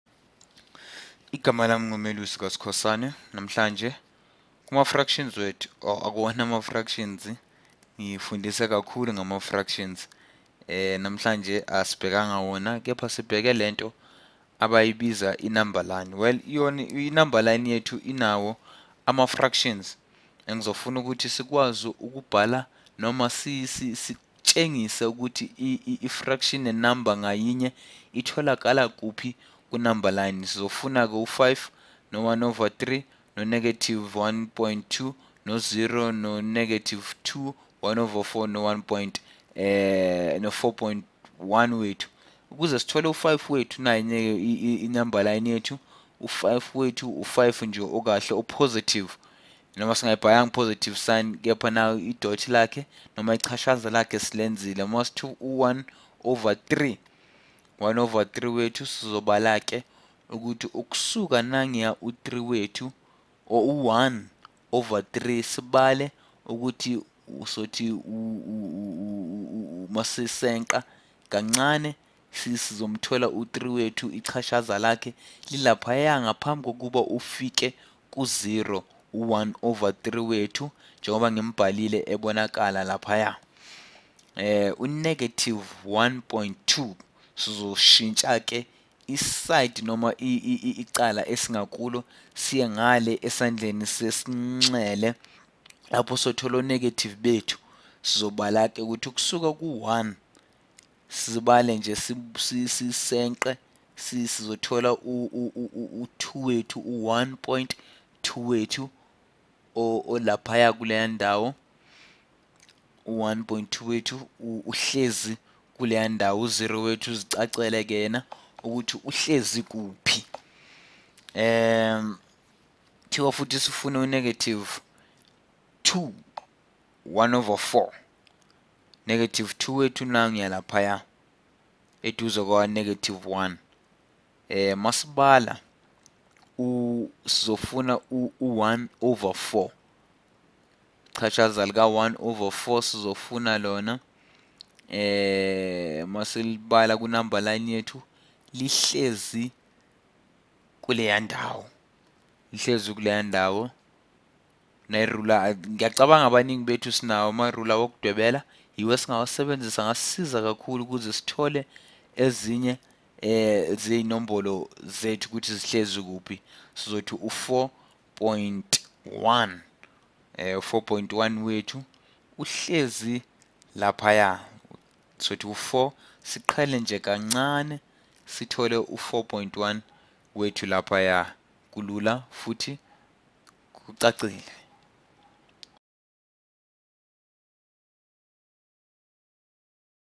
Points on a number line
This original Khan Academy video was translated into isiZulu by Melusi Skhosana. The translation project was made possible by ClickMaths